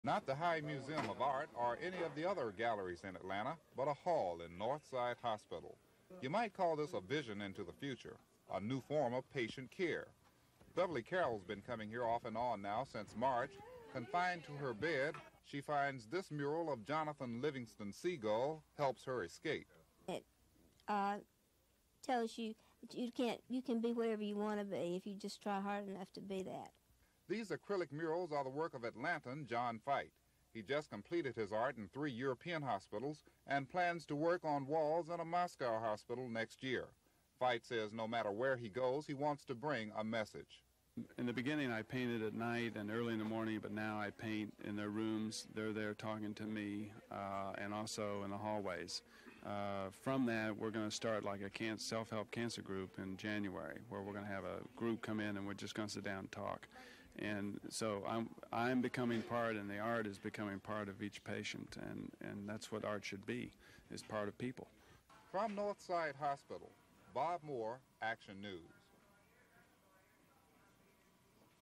Muralist John Feight Interview (December 2, 1979)
Murals grace the halls of Northside Hospital.  Hospital patient gets wheeled to see the art. Patient interviewed.  Interview with painter, John Feight, says the art is becoming part of each patient.
Reporter:  Moore, Bob
Hospital art tour gives hope to cancer patients
It all started in 1973 when John Feight volunteered to paint a wall mural at Northside Hospital. While working on the mural, a young patient asked Feight if she could help him paint. That gave Feight the idea that art can give comfort and love and ultimately planted the seed for the Foundation for Hospital Art.

 By Kroger ADVERTISER CONTENT
Can you ace this challenging Black history quiz?
The Atlanta-based nonprofit devoted more than 30 years to bringing comfort to hospital patients by transforming sterile hospital walls with vibrant paintings created by patients, their loved ones and hospital employees.

“Hospitals have been places so focused on providing care and diagnoses, that they often lose sight of providing an environment conducive to healing. We believe that art can play a big role in healing. Art can transform a person’s attitude. It can provide hope,” said Scott Feight, executive director.

To spread hope through art, the foundation kicked off its first “PaintFest America” tour stopping at cancer facilities in 50 cities in 50 days. During each PaintFest America event, participants will paint paneled canvas murals in one cancer treatment hospital per city per day

The tour stopped at Atlanta’s Winship Cancer Institute of Emory University on July 12. Patients and survivors came together to paint murals for their hospitals, and one special state-specific panel that will be incorporated into a larger, 50-state “Stars of Hope” mural to be unveiled in New York on the final tour stop.

“Winship Cancer Institute was honored to be representing Georgia for PaintFest America and the Foundation for Hospital Art’s efforts,” said Diane Cassels, chief administrative officer ofWinship Cancer Institute of Emory University. “It was important to bring patients, caregivers and healthcare professionals together because we are not just there to treat their cancers, but we also care about their quality of life and we are a place that can provide synergy for living with cancer.”

With more than one million volunteers, the Foundation for Hospital Art has donated 44,000 paintings to more than 4,000 health care facilities located in 195 different countries.

Foundation for Hospital Art is always seeking volunteers to continue spreading hope through art by offering PaintFest kits to corporations and individual organizations interested in hosting their own event.

“The day of cancer diagnosis is one of the darkest days of anyone’s life,” said Feight. “Our communities need our hospitals to be places that are comforting and offer hope, and we believe that art is a way to do this.”